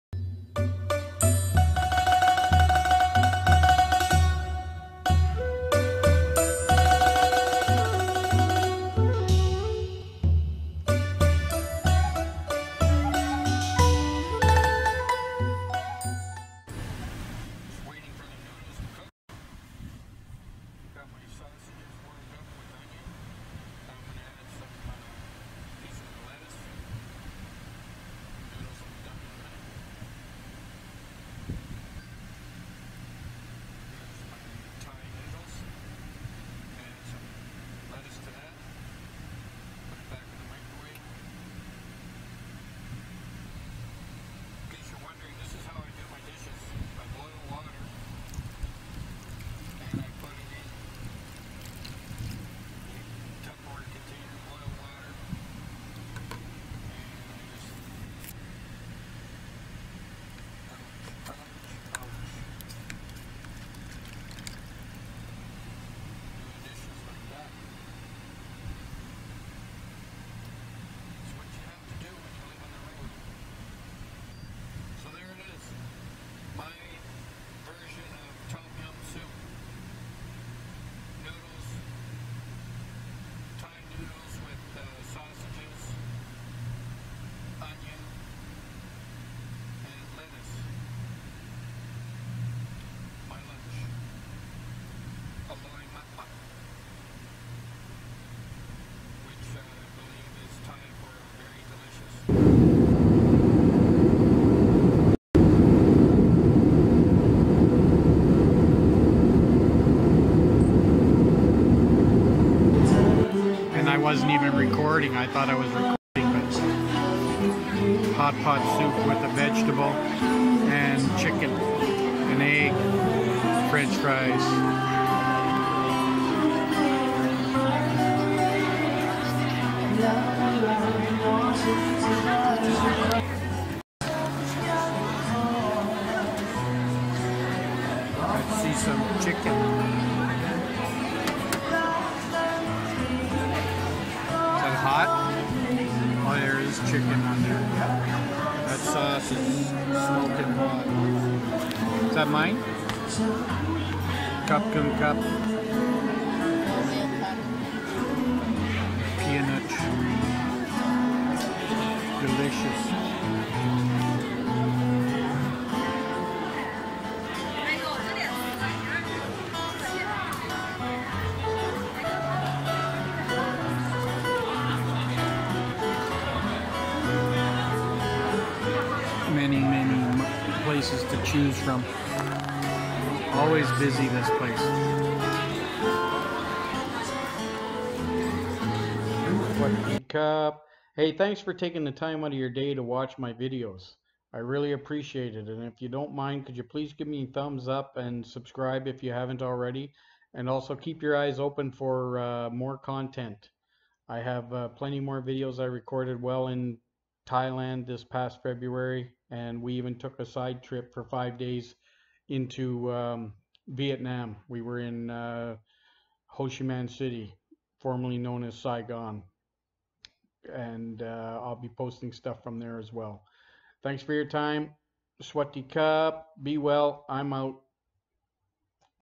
My Road to Thailand pt4
On this video of My Road to Thailand, I start off cooking in the truck and end with one our favourite places to eat in Chiang Rai Thailand. BTW The audio in the truck is terrible. Sorry about that it's only for a little bit.

I can't Believe how 5 months flew by so quickly."